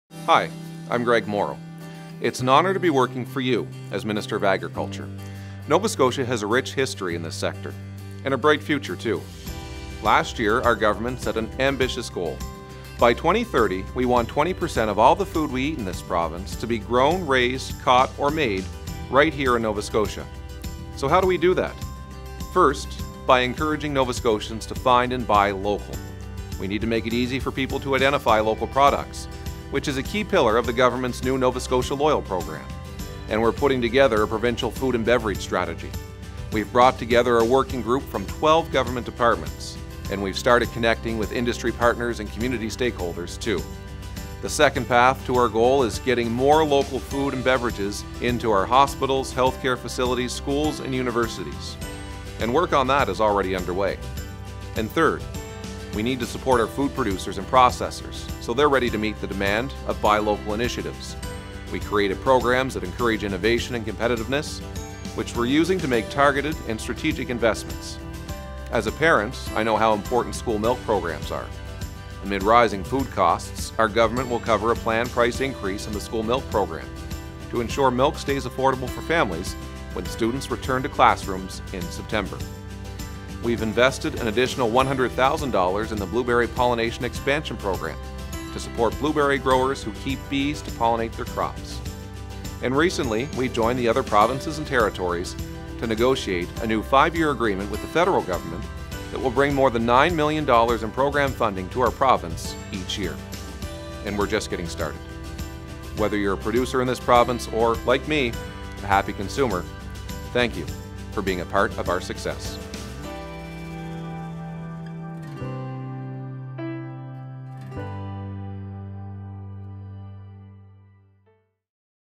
Department of Agriculture - Minister Morrow
"Nova Scotia has a rich history in this sector – and a bright future, too."

Minister Greg Morrow reflects on his first year at the Department of Agriculture.